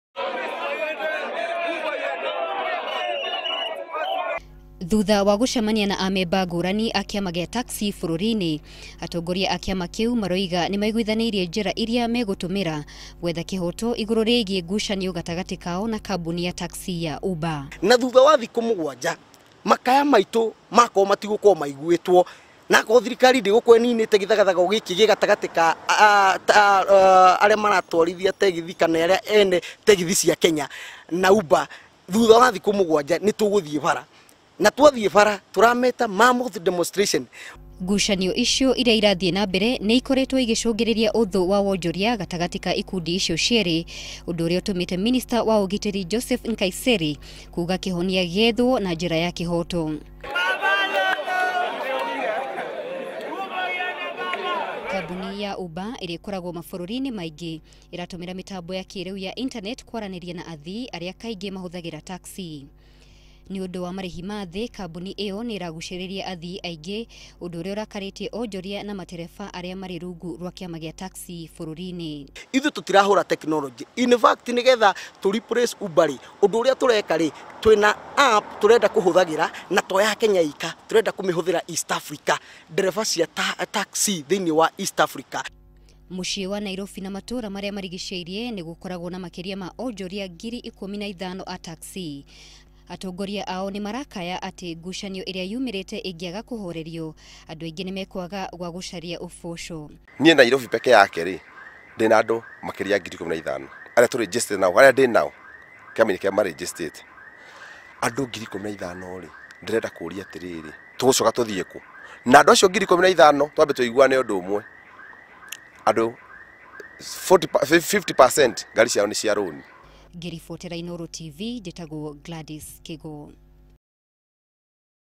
Ngucanio cia ukuui
Ene taxi kuruta mukana kuri thirikari wigii ngucanio yao na kambuni ya uber